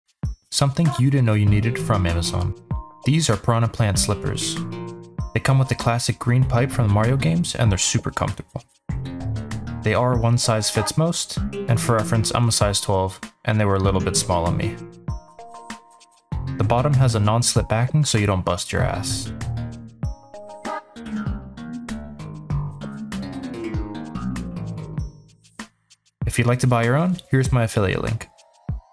You NEED Piranha Plant Slippers
I find unique products on Amazon that you didn't know you needed. I reviewed Piranha Plant slippers from the Mario games.